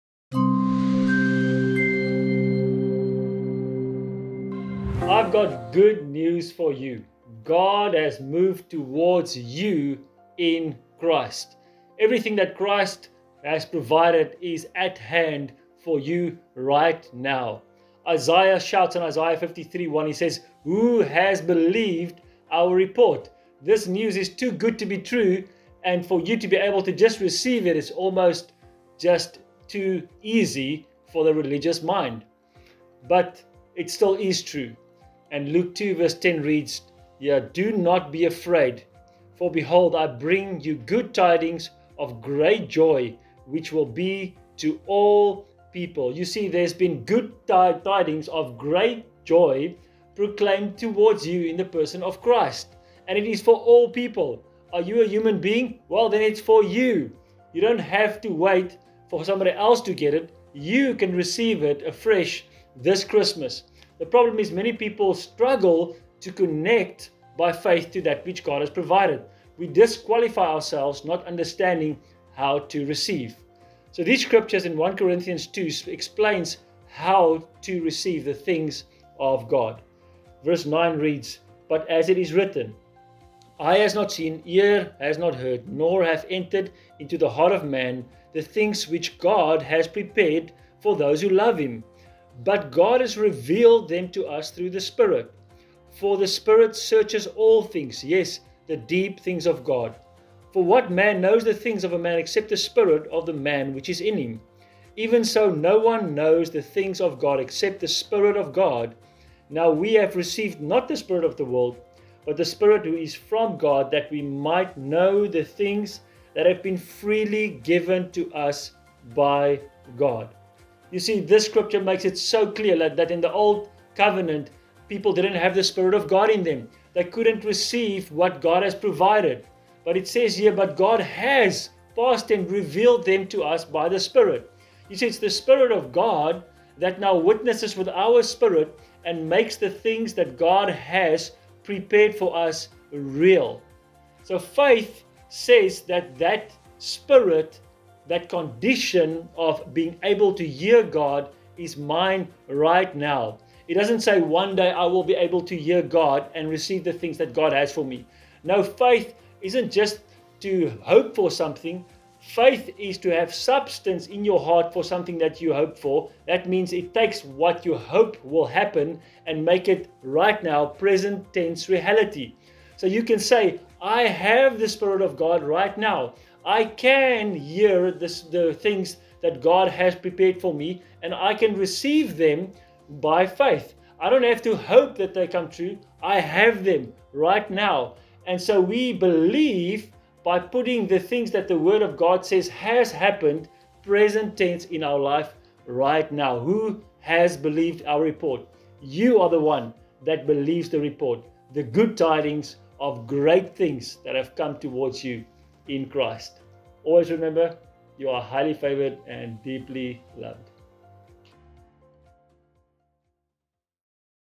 Devotions - Who has Believed? - 20 Dec Ps Alfred Rademan (Part 2)
Good news for you - God has moved towards you in Jesus
Isaiah 53 v 1
Luke 2 v 10
Good tidings of great joy are for you
1 Corinthians 2 v 9-12
I have the spirit of God in me right now
I believe